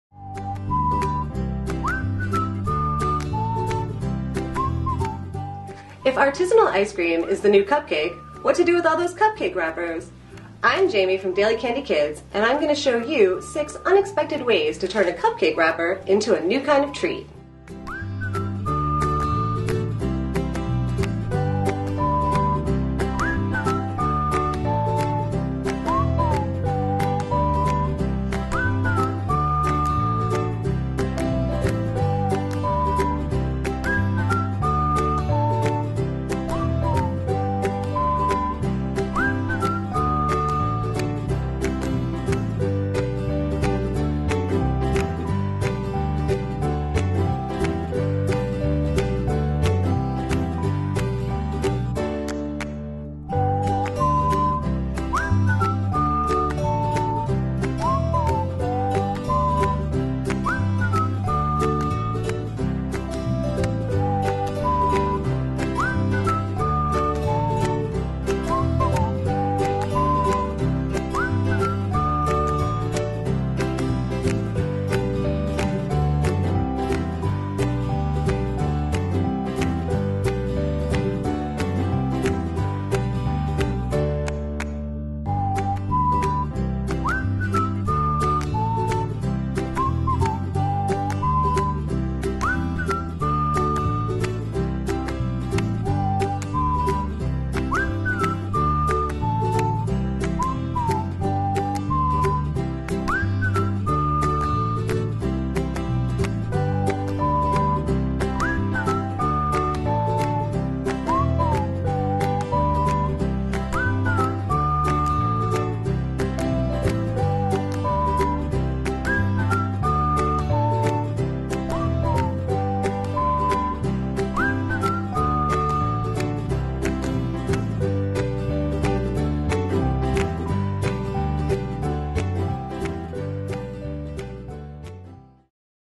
The Secret Lives of Cupcake Wrappers
We've found six ways to turn the humble, pleated paper cup into your party's main attraction. From ice cream bowls and name card holders to garlands and flowers — it's a cupcake wrapper celebration you'll eat up.